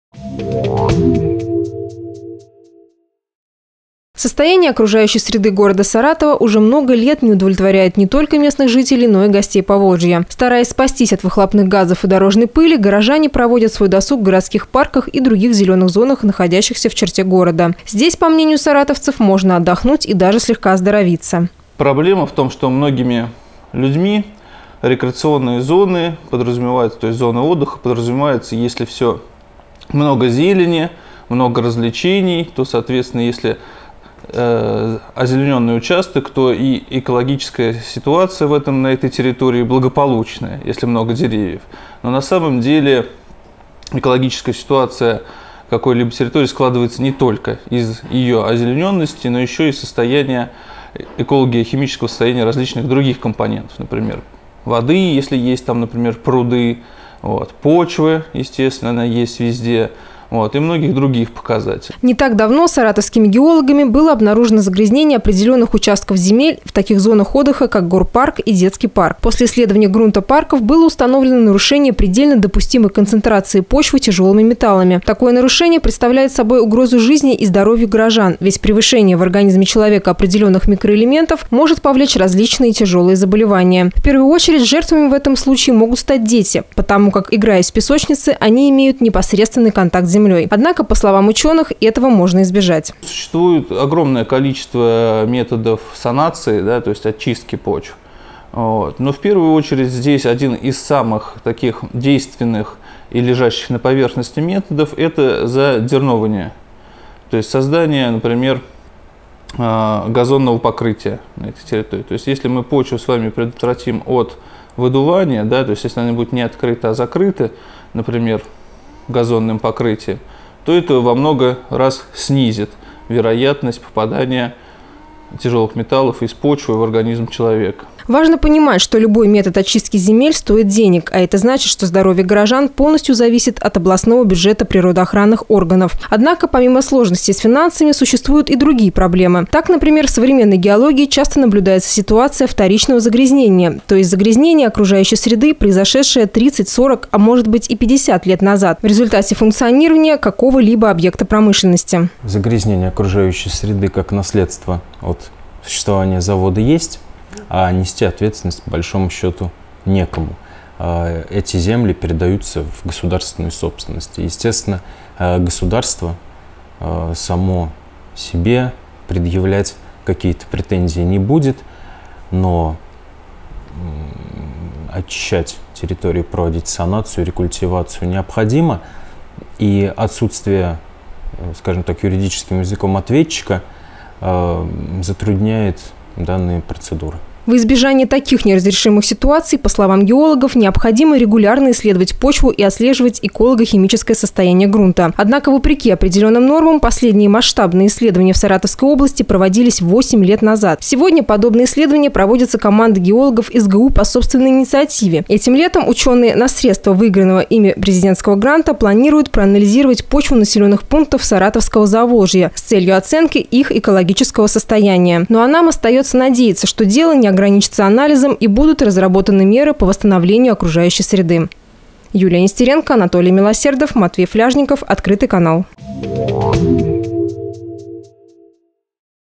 Грязная почва в саратовских парках
В саратовском Городском и Детском парке местными геологами была обнаружено высокое содержание в почве тяжелых металлов.